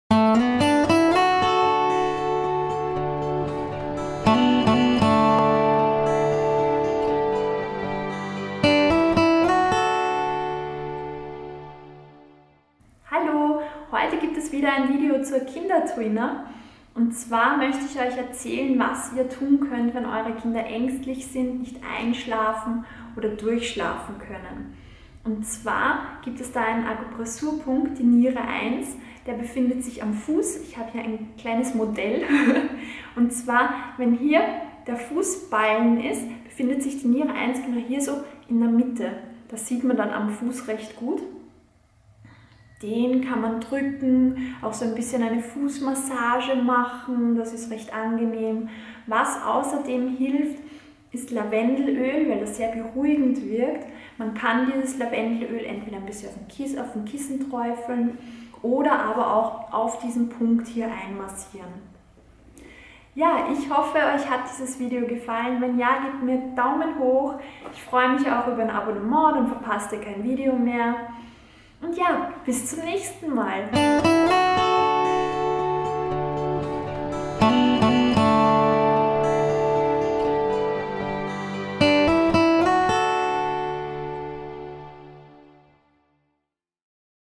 TCM Akupressur: Kinder Tuina bei Angst und Schlafstörungen
Wenn du auch deinen Sexy Spirit Lifestyle finden willst, dann werde doch gleich VIP

Dieses Kinder-Tuina-Rezept der chinesischen Medizin (TCM) kannst du anwenden, wenn dein Kind ängstlich ist, nicht einschlafen oder durchschlafen kann.

Die Kinder-Tuina wirkt natürlich auch bei Erwachsenen.